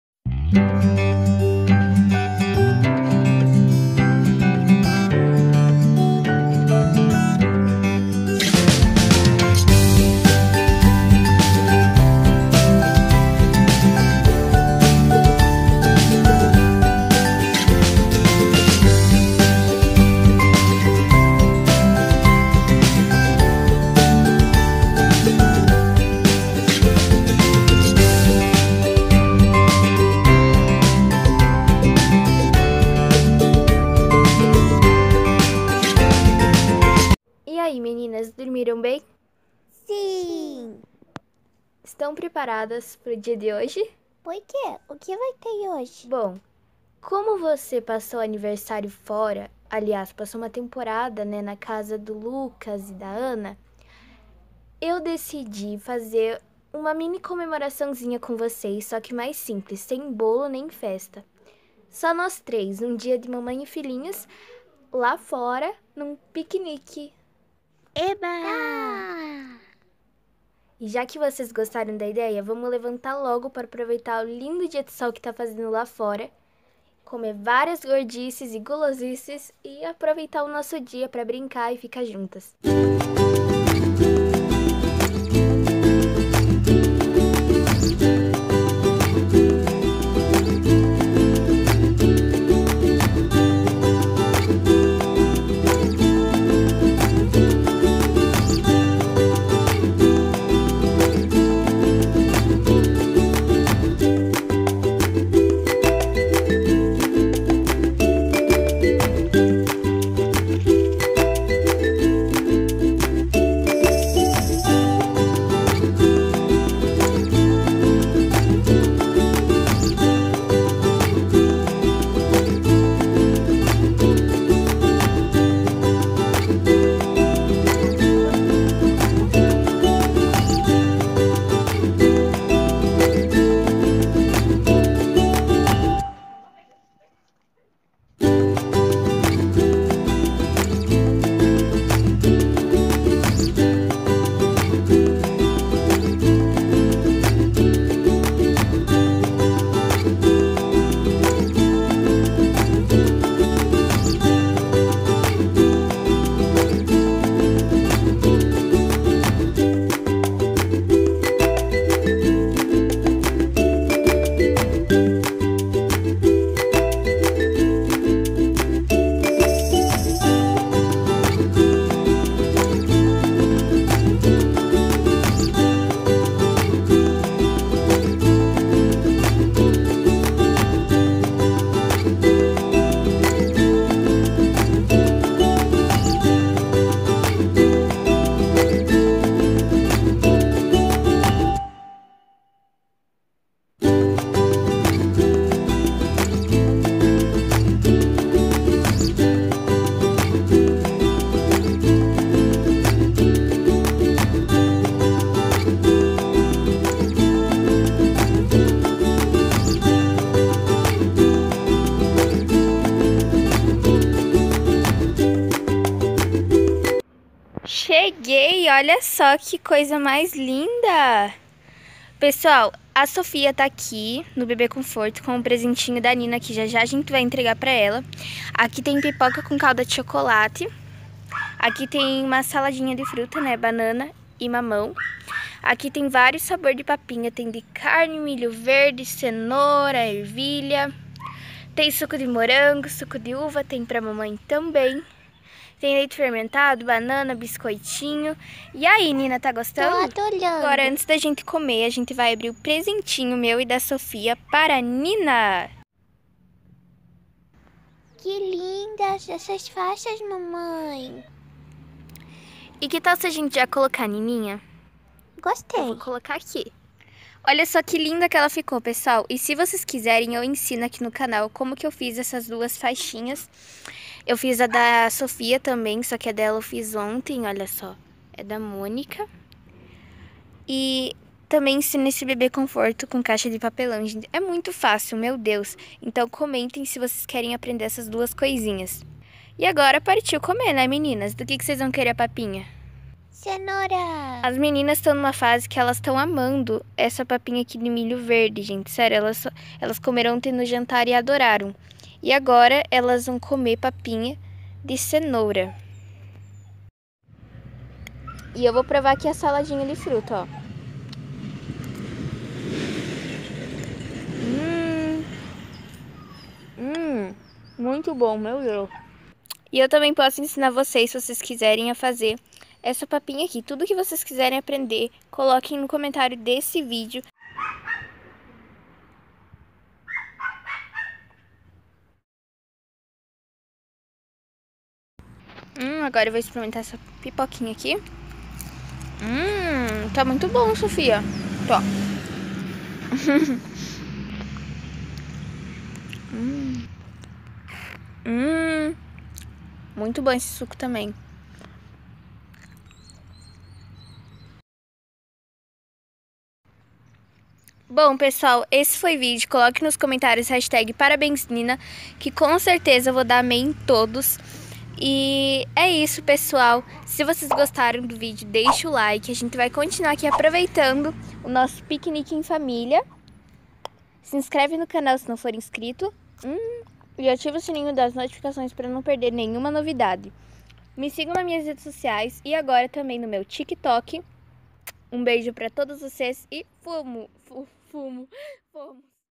ESPECIAL 3 ANOS DA NINA: TARDE DE PIQUENIQUE COM AS BEBÊS!!
Hey amorees💕Como vcs estão??💕
Neste vídeo mostrei como foi a comemoração do aniversário da Nina Espero que gostem😘

       Fuiiiii...